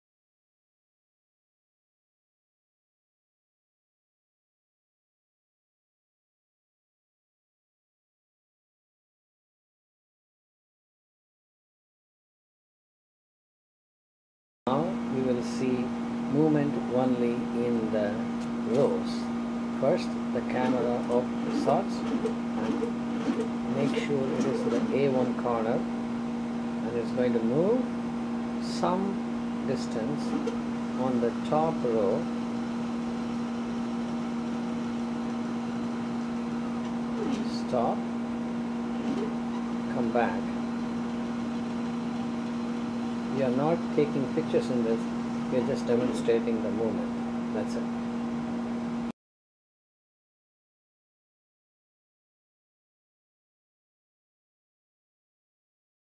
SRI Homing and Right and Left
This demonstrates the Simple Robotic Imager for the protein crystal screening. In this demo we are showing homing of the imager, camera function, right and left movement. No image recording is shown.